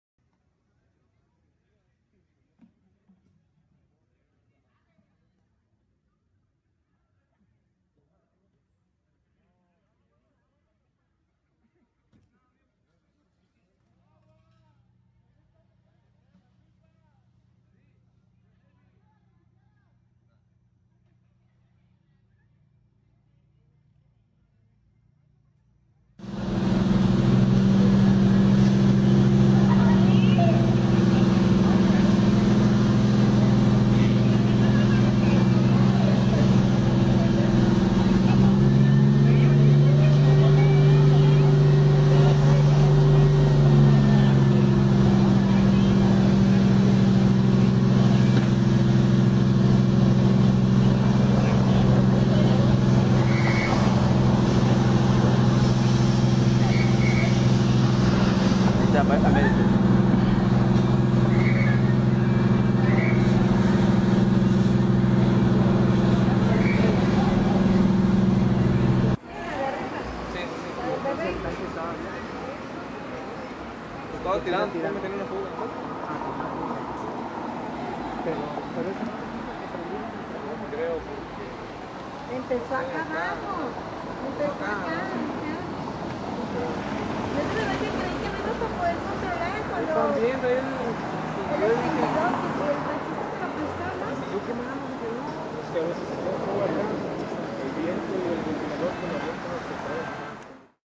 Por falla mecánica se incendia auto en avenida Ignacio Sandoval, en Colima
Colima.- La noche de este viernes 11 de septiembre se registró el incendio de un vehículo tipo monza, color gris, sobre la avenida Ignacio Sandoval, entre las calles Juan José Arreola y Juan Rulfo en la ciudad capital del estado.